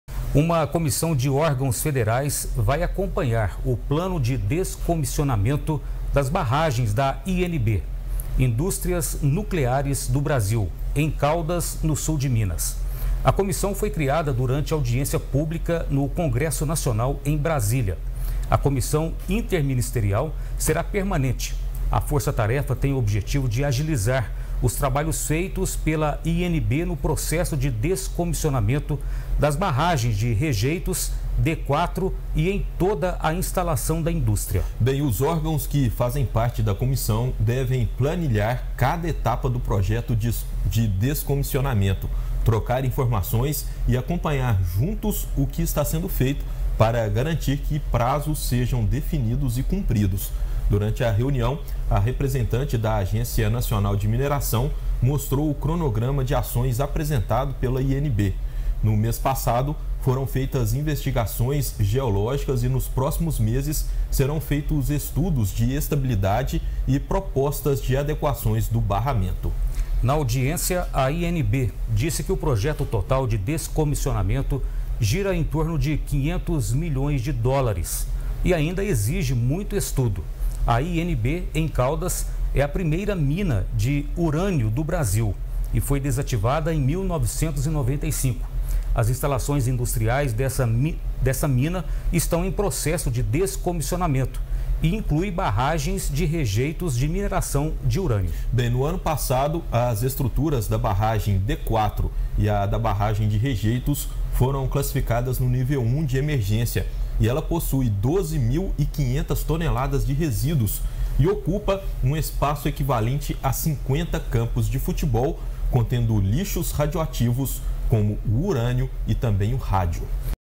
COMISSÃO FEDERAL É CRIADA PARA ACOMPANHAR BARRAGEM DE MATERIAL RADIOATIVO COM RISCO DE ROMPIMENTO
| REGIÃO |

Uma comissão de órgãos federais vai acompanhar o plano de descomissionamento das barragens da INB (Indústrias Nucleares do Brasil), em Caldas, no sul de Minas. Saiba todas as informações: